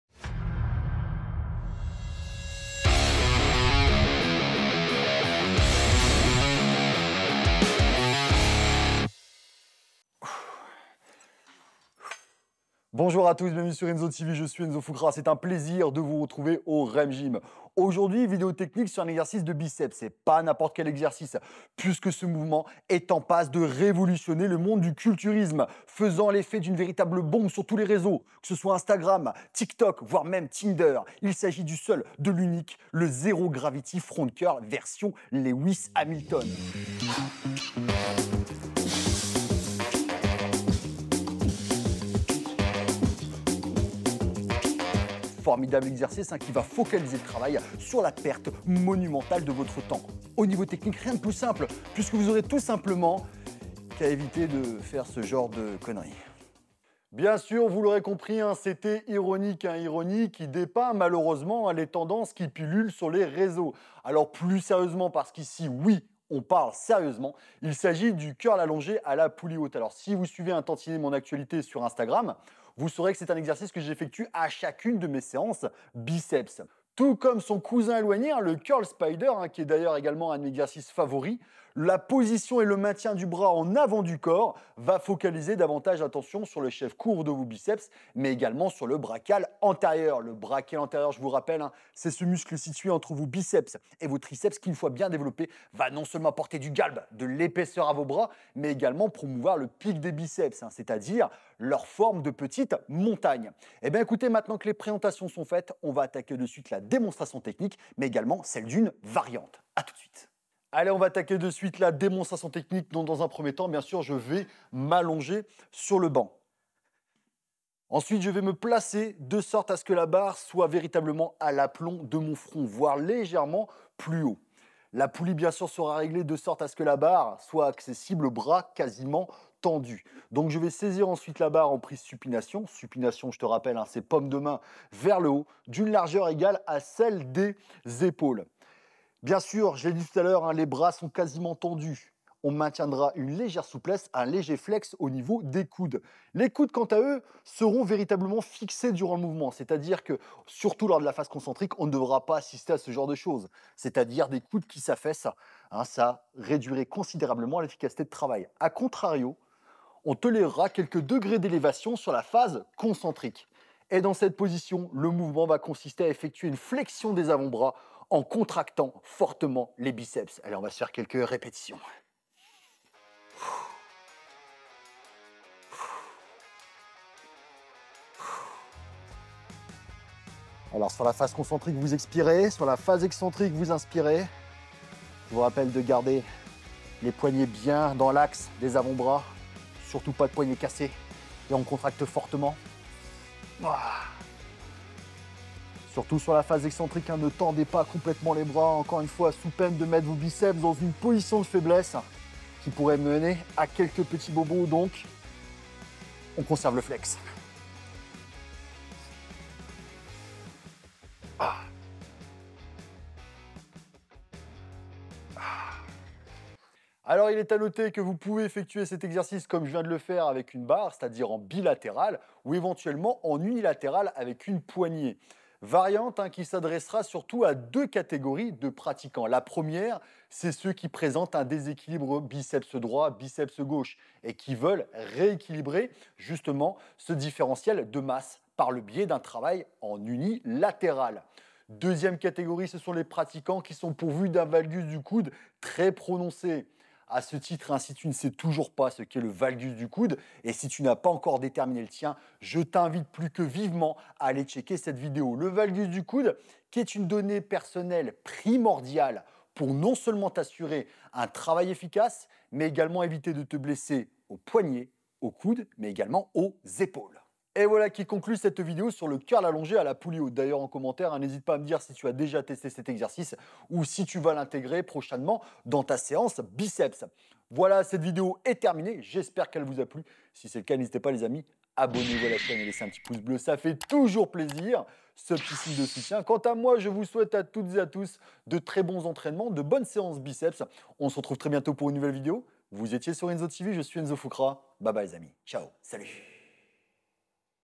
DES BRAS + MASSIFS AVEC LE CURL ALLONGE !!! (+ VARIANTE)
Dans cette vidéo YouTube sur la musculation , je vous propose la démonstration technique d'un exercice biceps, le curl allongé à la poulie haute

Merci à mon caméraman Pierre Ballat pour son grand professionnalisme